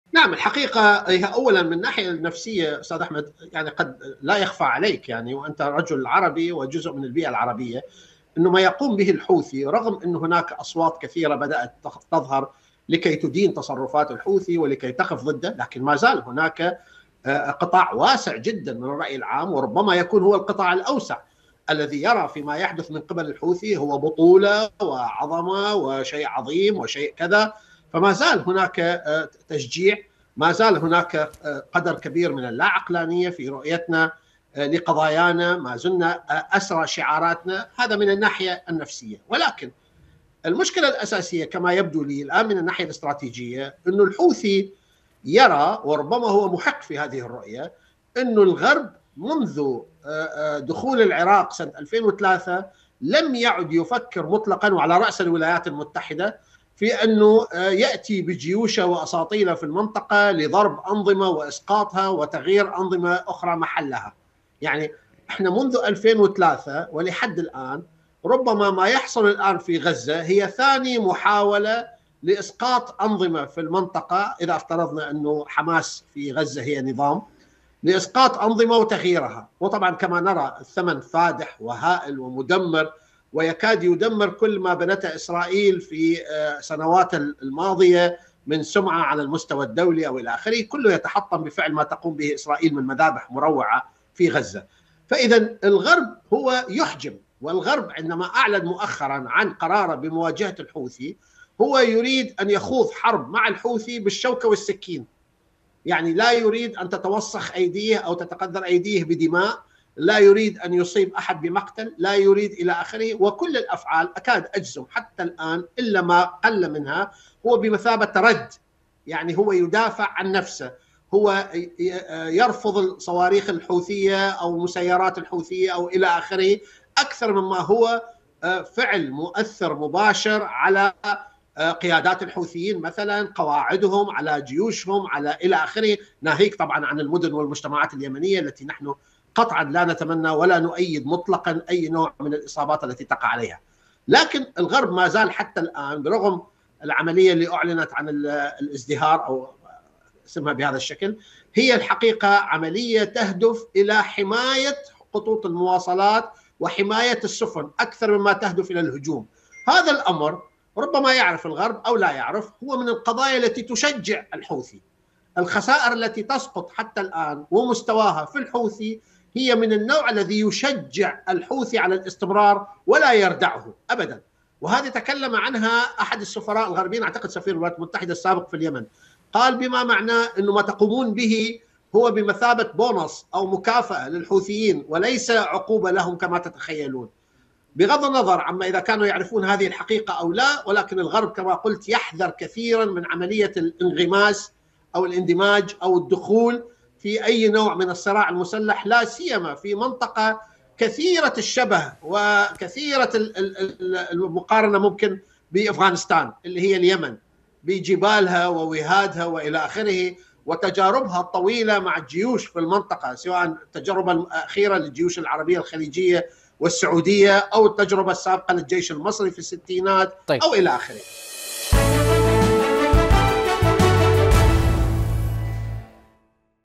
دريد: لهذه الاسباب لم تتمكن واشنطن من ردع اليمن
موقع قناة التغيير الفضائية  الالكتروني